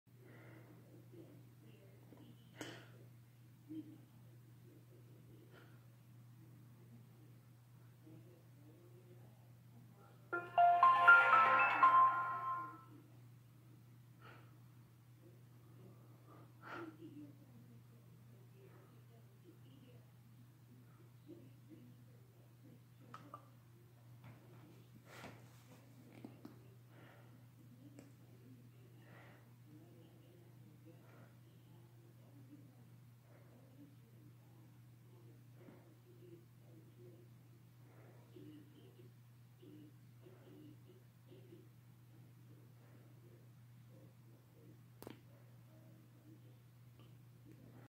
Galaxy S5 Vs Note II
Galaxy note II has 16gb of space
brand verizon unlocked
os 4.4.2 kitkat

galaxy s5 has 6.0.1 marshmallow os
16gb space
brand verizon USA unlocked too cdma and gsm